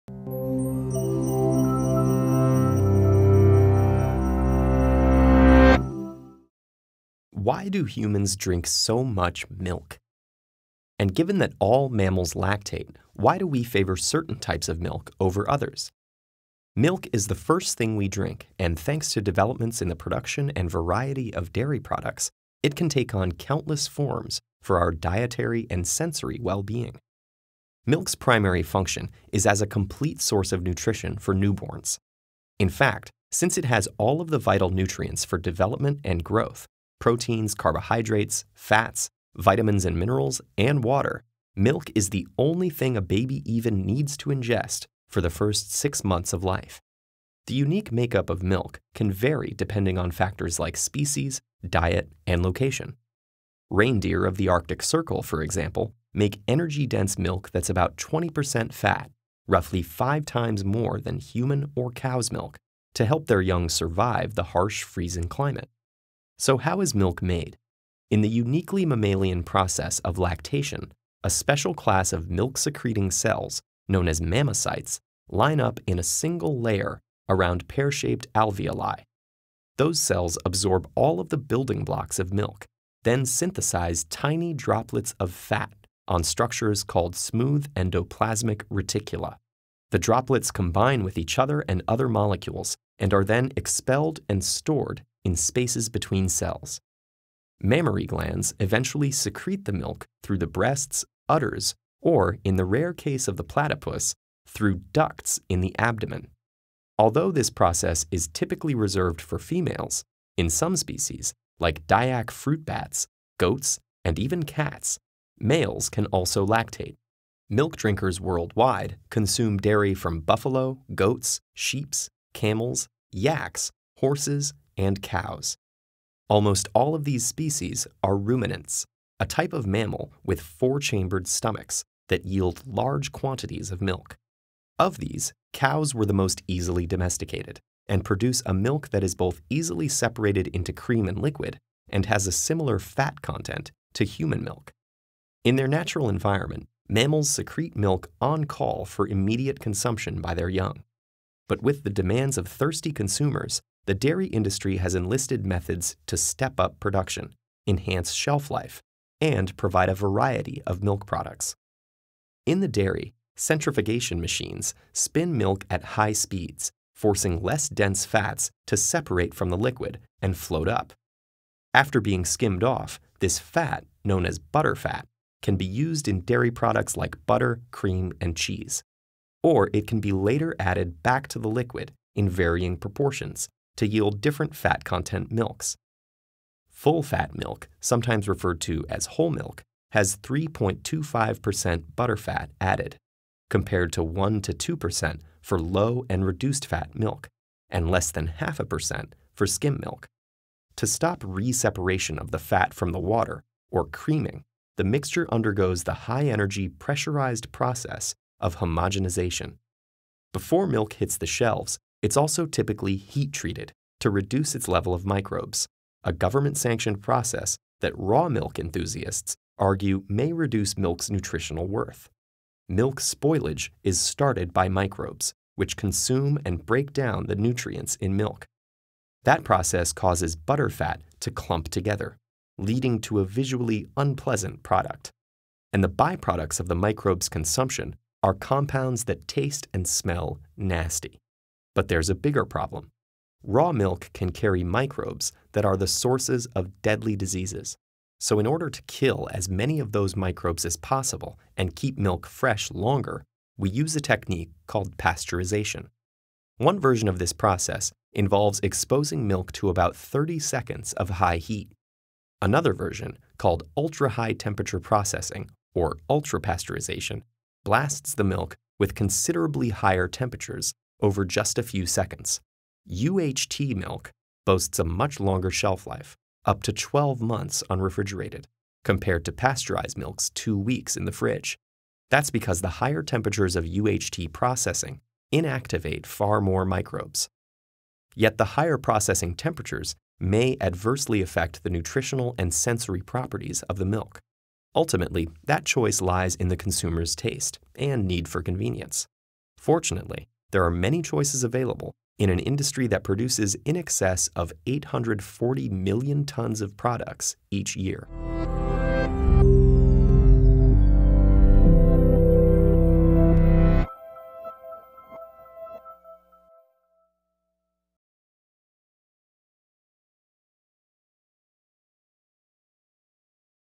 The science of milk - Jonathan J. O'Sullivan
The milk industry produces in excess of 840 million tons of products each year. Why do humans drink so much milk? And given that all mammals lactate, why do we favor certain types of milk over others? Jonathan J. O’Sullivan describes how milk is made.

Lesson by Jonathan J. O'Sullivan, animation by TED-Ed.